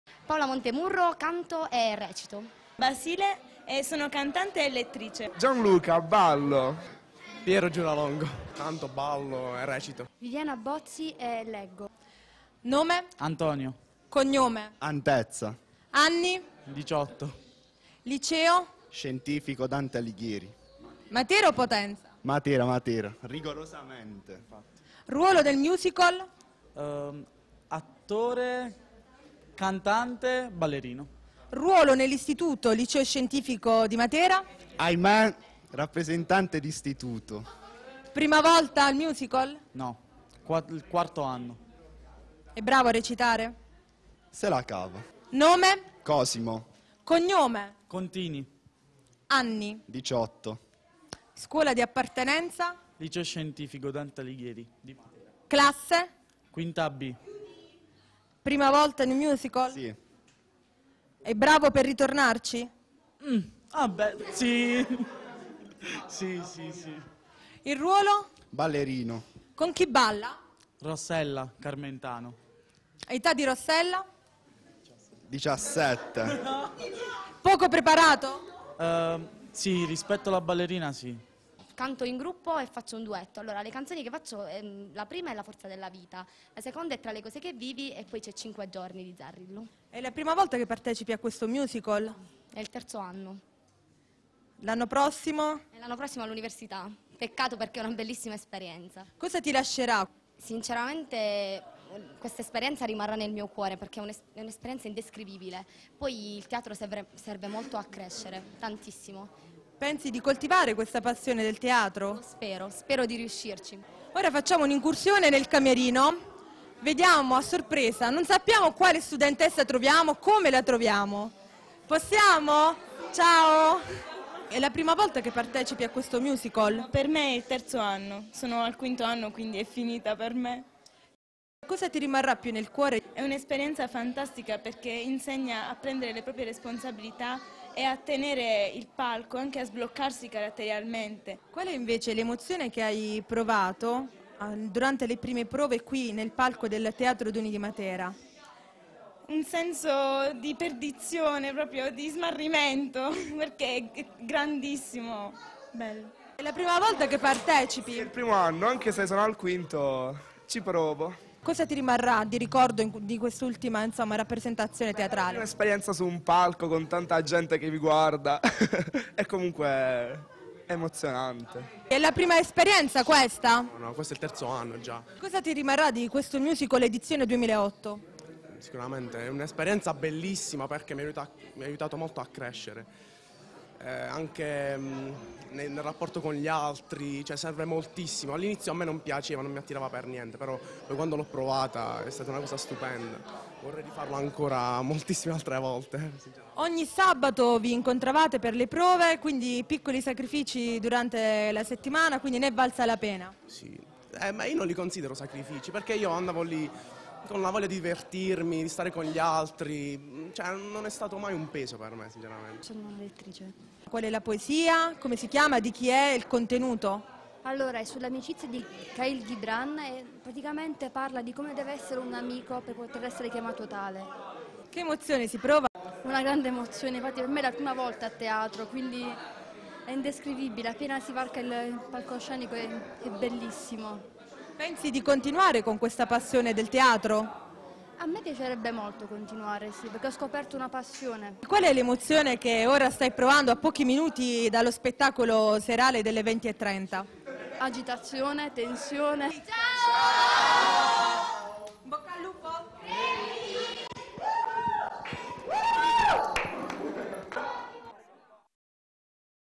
Liceo Scientifico di Matera presenta Levia Gravia
Matera 21 aprile 2008 - Lo spettacolo è stato reralizzato dagli alunni del Liceo Scientifico "Dante Alighieri" di Matera, aiutati e sostenuti dal preside Carnovale e dagli insegnanti. "Levia Gravia" è un insieme di ballo, canto e poesia.